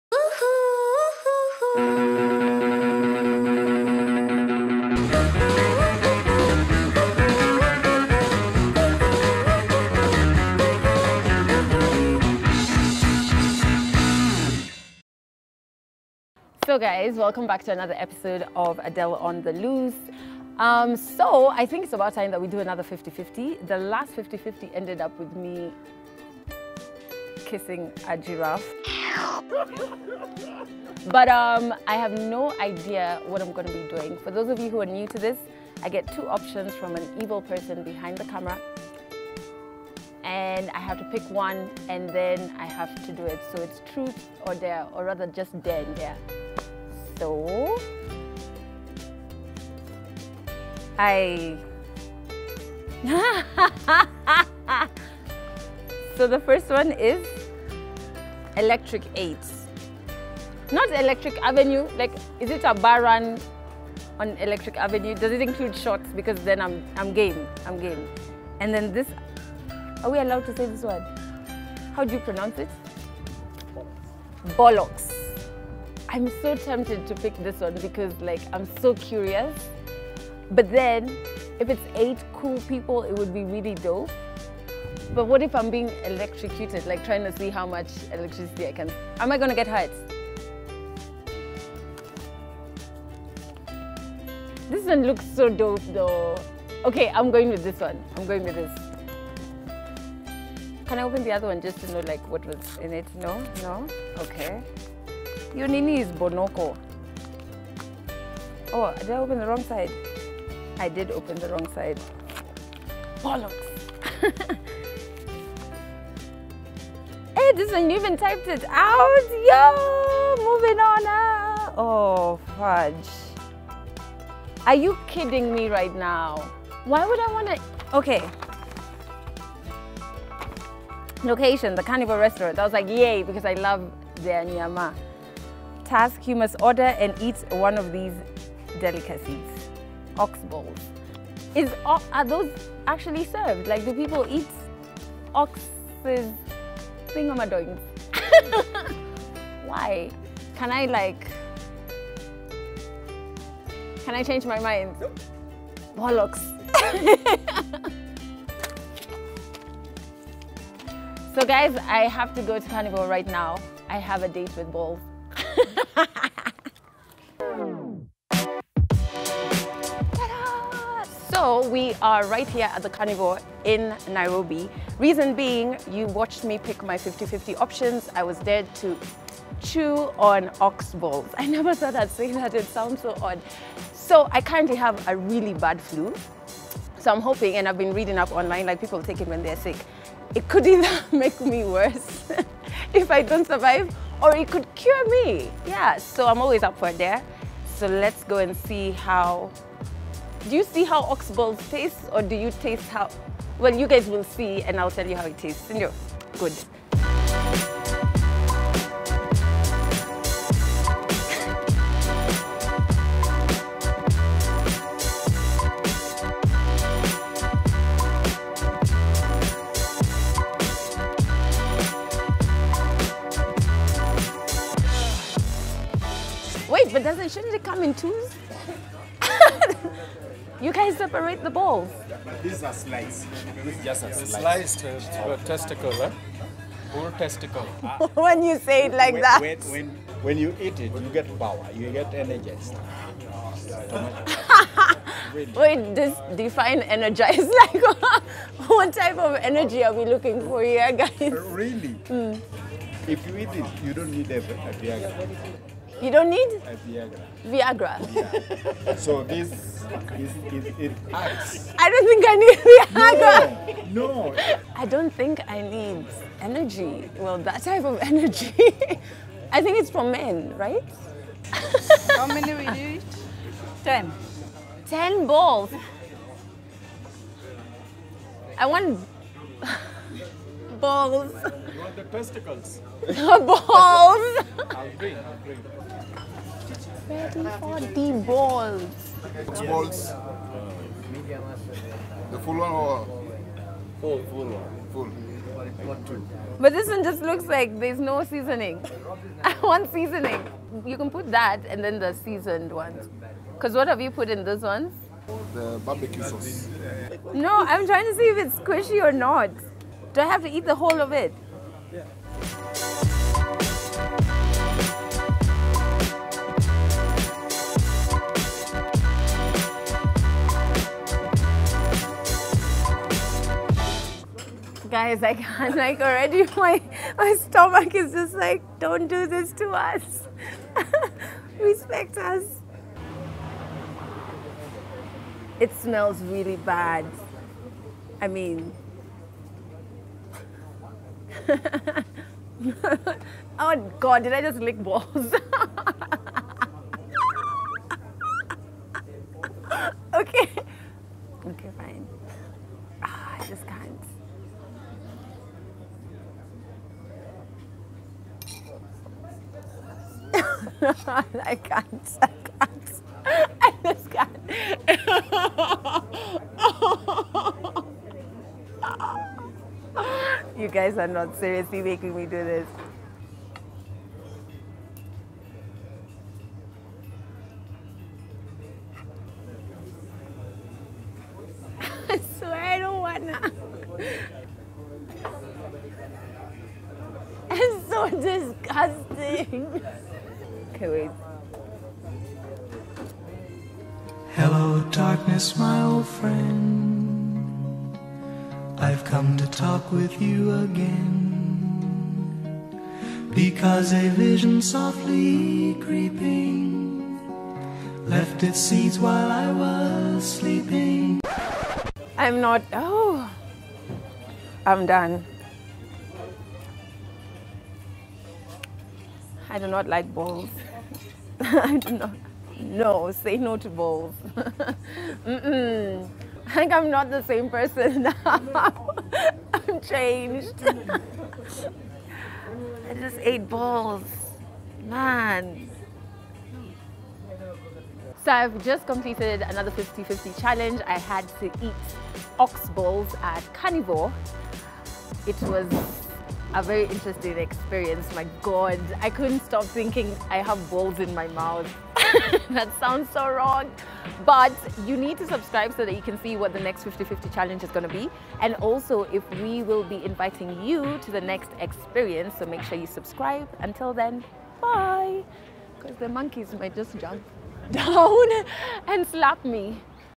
Another 50/50 | Adelle On The Loose Ep9
Another 50/50 challenge on AdelleOnTheLoose - this time I was dared to eat things that: Come in pairs and hang down below!

Press play to find out what!!!

Shoutout to CARNIVORE KENYA for hosting my madness!